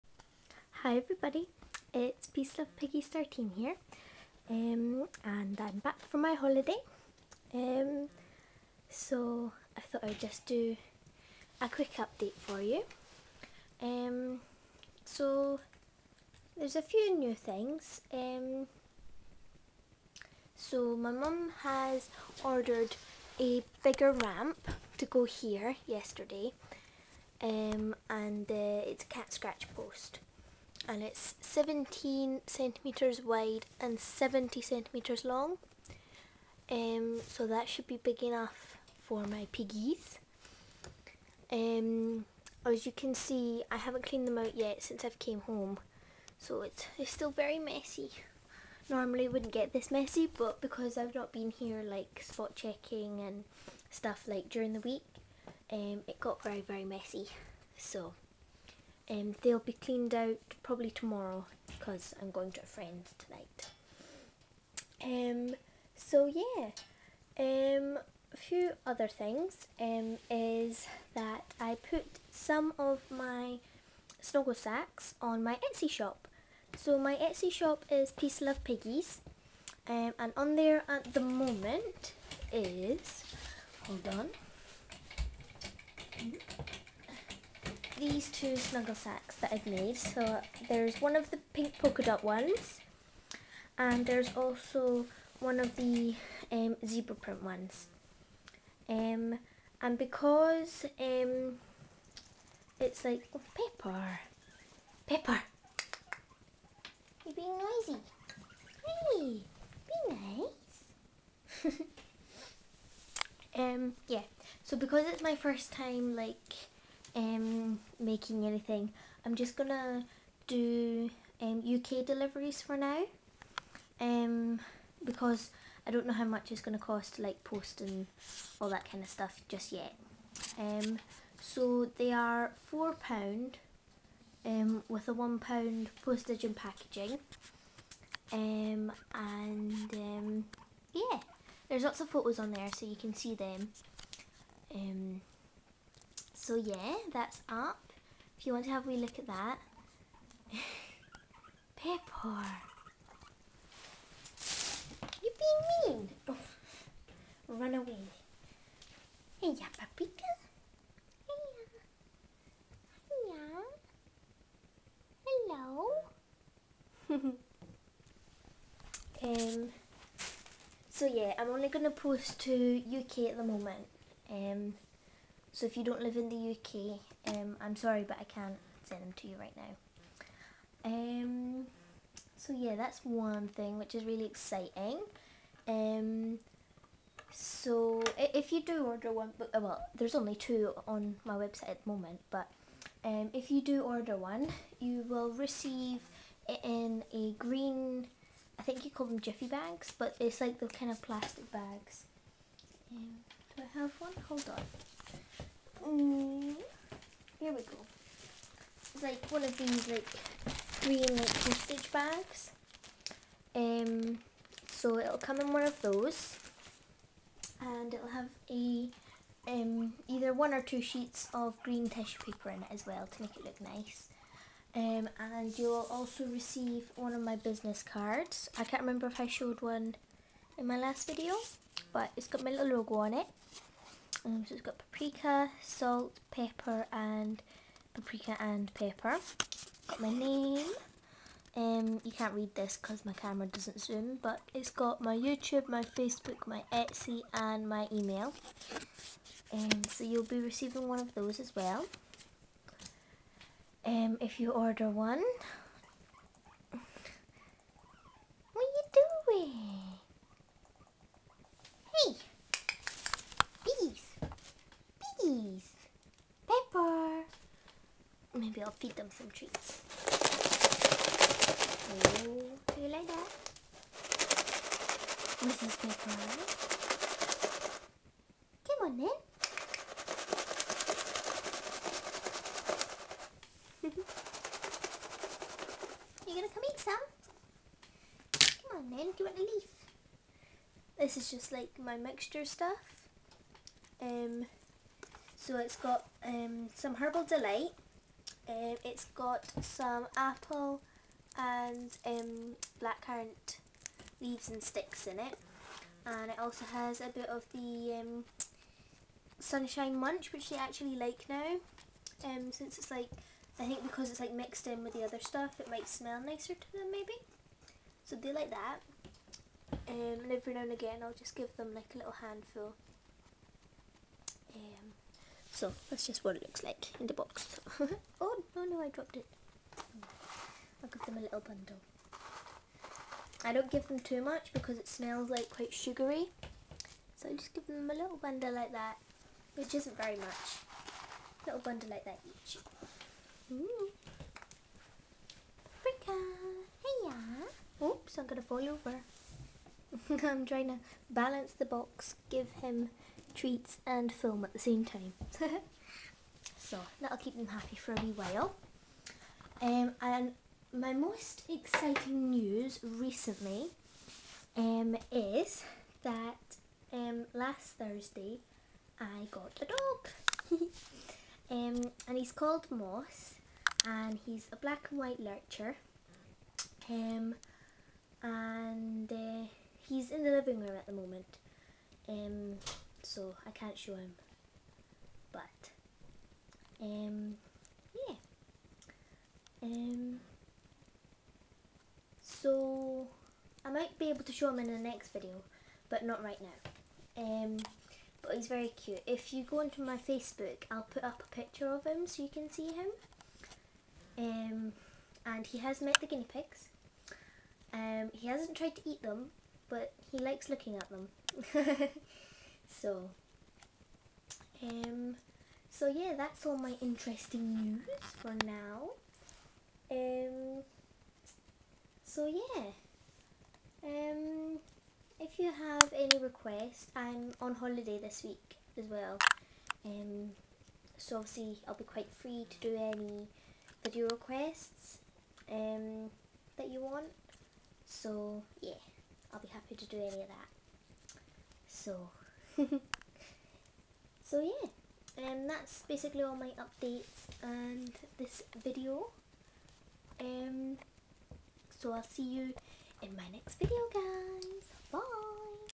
Im Back, With Some Surprises!
I am back from my holiday and thought I would quickly update you guys on whats new in Piggy World! I will do a video all about my new dog next time! :)